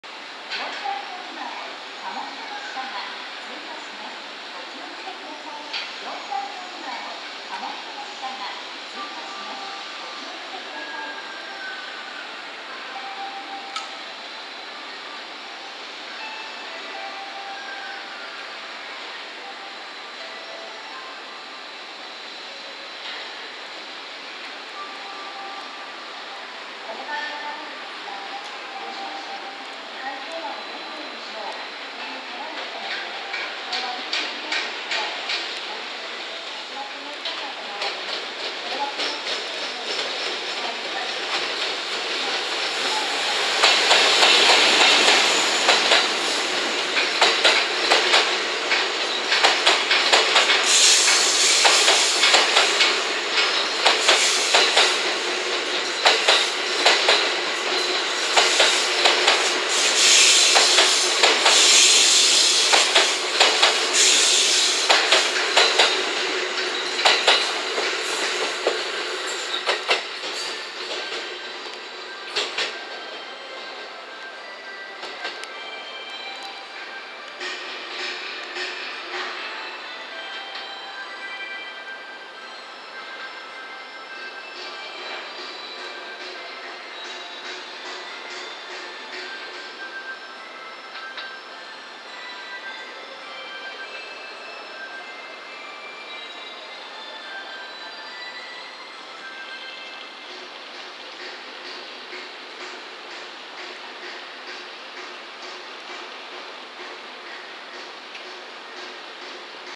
4号車食堂車 サシ 山陽本線 上り 甲種輸送 8862レ EF210-6号機 牽引 E261系 サフィール踊り子10B（RS1 RS2編成）10両 日立製作所 JR貨物 2019.11.6 01175
山陽本線 上り 甲種輸送 8862レ EF210-6号機 牽引 E261系 サフィール踊り子10B（RS1 RS2編成）10両 日立製作所 笠戸 下松 JR貨物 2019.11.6

川重：1-3号車 　日立: 4-8号車
RS1 川重(19.11.5) 日立(19.11.6)
RS2 川重(19.11.5) 日立(19.11.6)